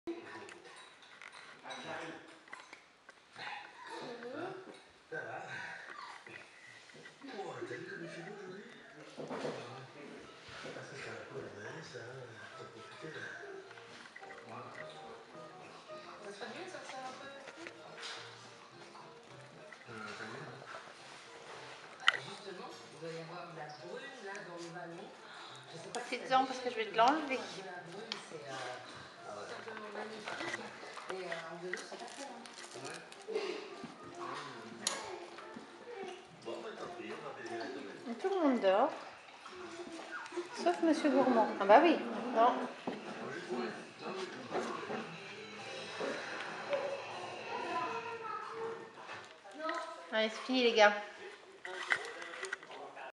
Portée M J + 33 , chiots de 5 semaines au BARF
Maman helfie a l'air de penser que je ne vous donne pas assez, alors le soir, elle va pleurer pour vous rejoindre et elle regurgite un petite partie de son repas.
Ce soir une dorsale de dinde a echappé à ma vigilence, et qui je trouve en train de grignoter son petit morceau tranquille pendant que les autres dorment...

Mr coccinelle est vraiment un sacré malin !